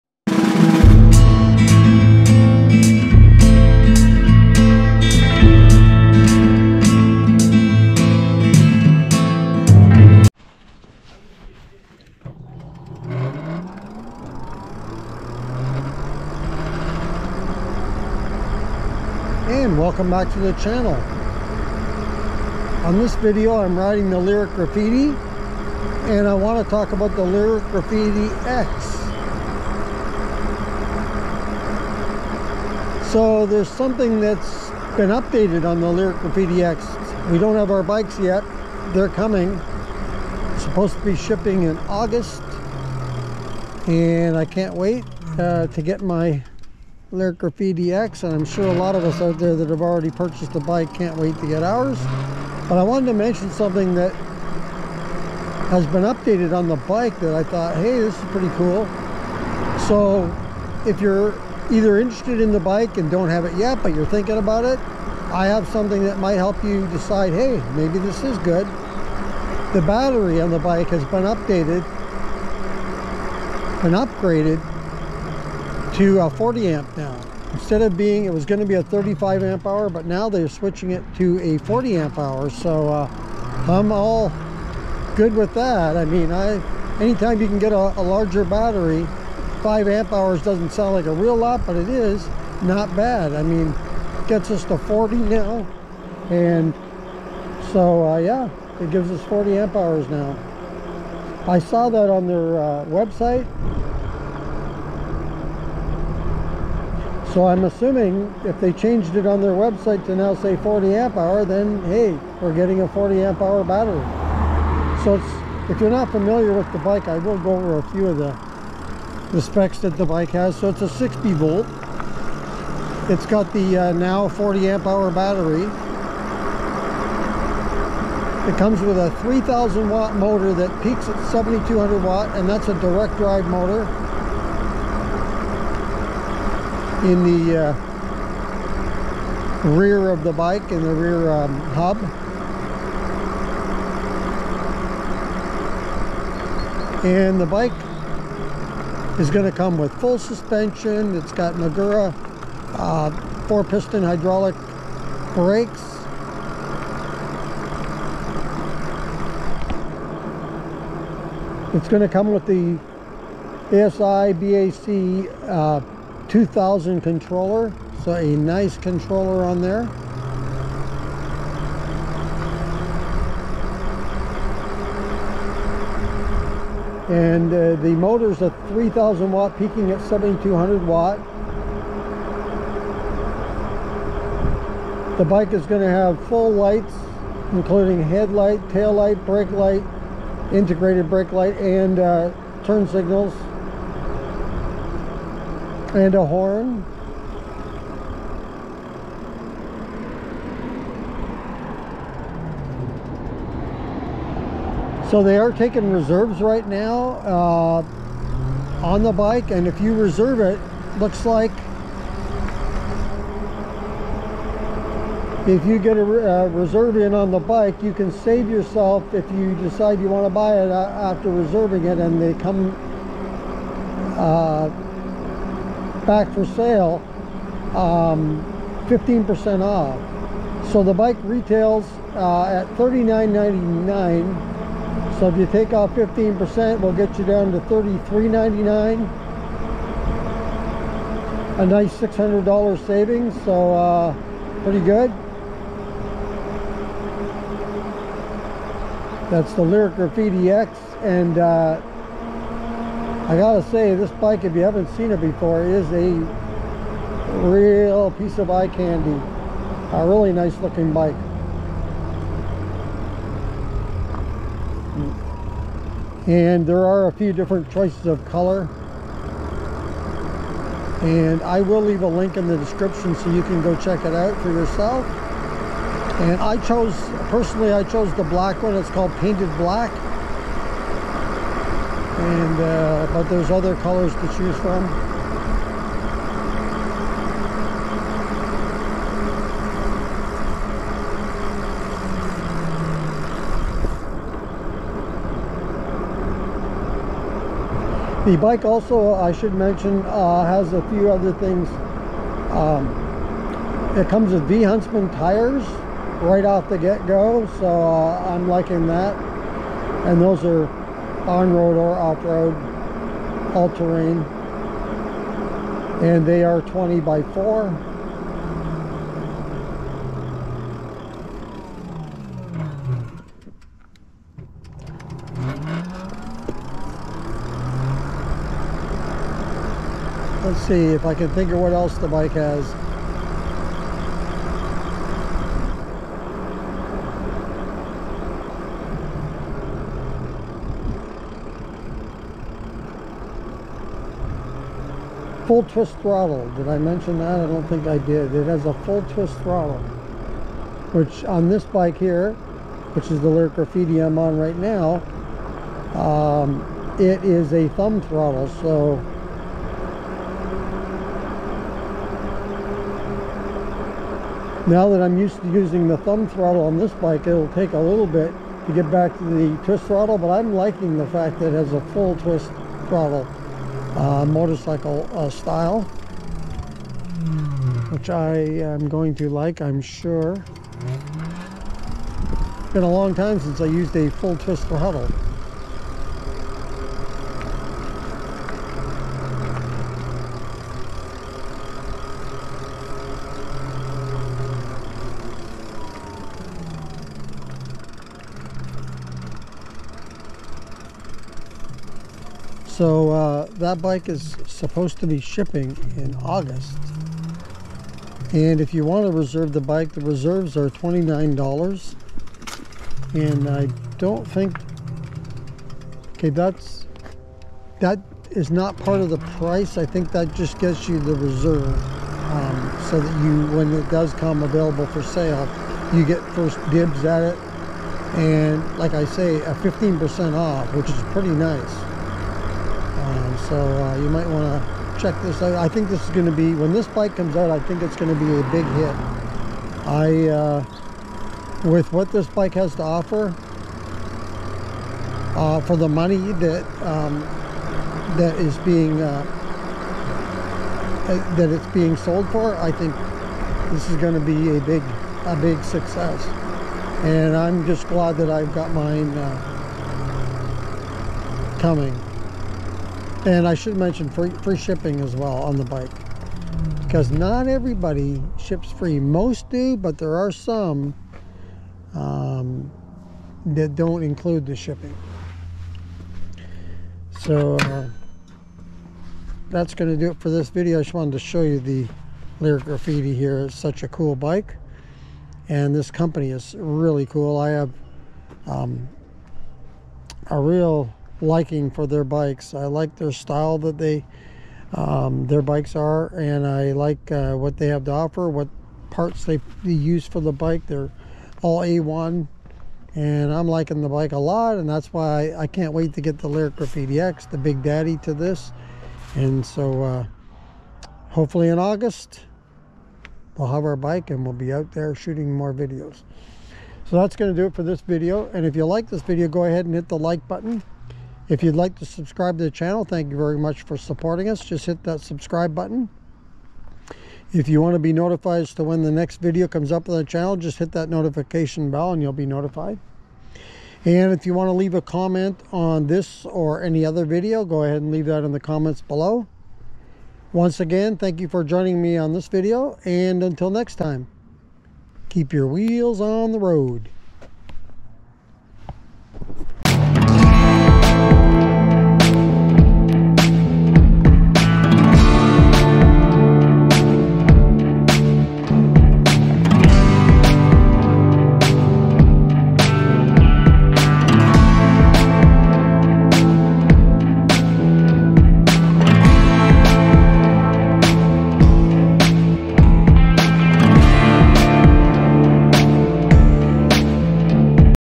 Lyric Graffiti X Upgraded Battery To 40ah
Talking About The Graffiti X Including The Battery Being Upgraded To 40ah - Riding The Lyric Graffiti